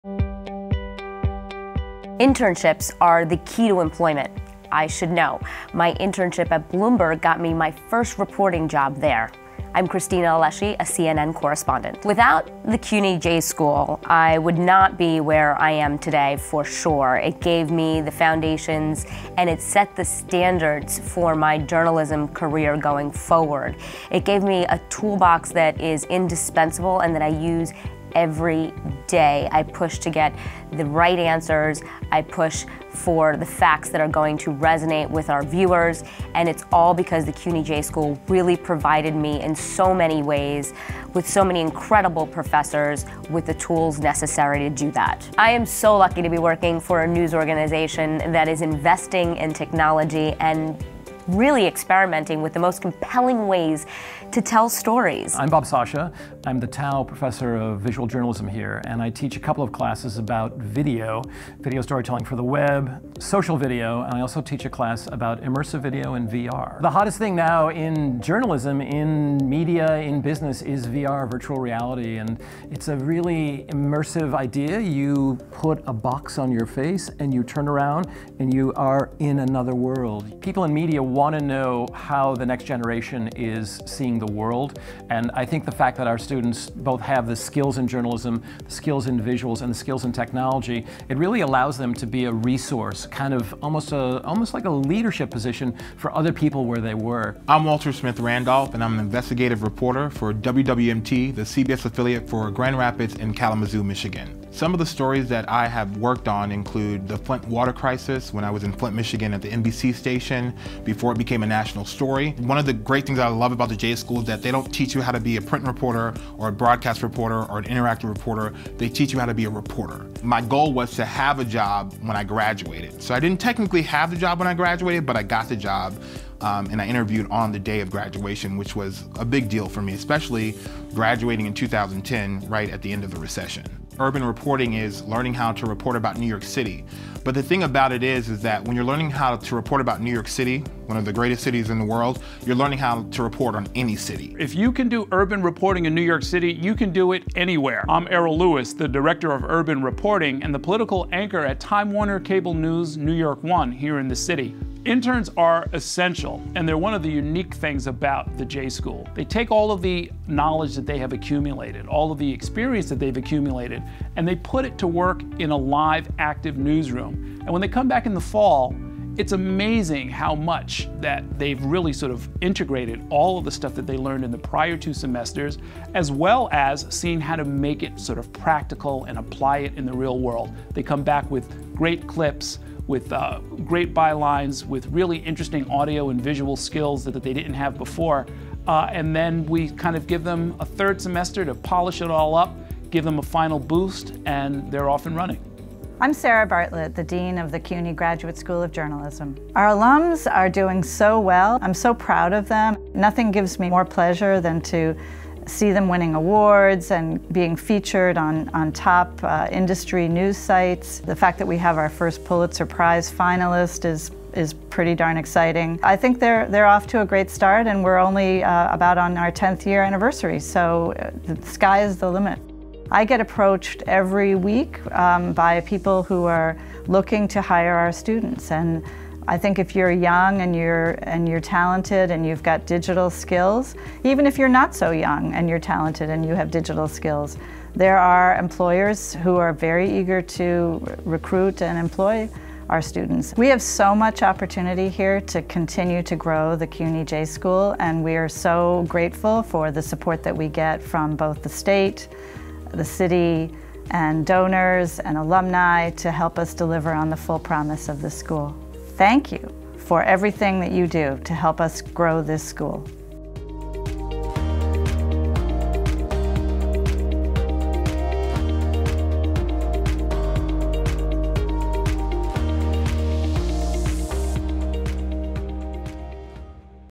CUNY J School Gets the Job Done
Merely 9 years old, the CUNY Graduate School of Journalism is already a leader in the field, training new journalists in the skills and tools they need to land a job and succeed.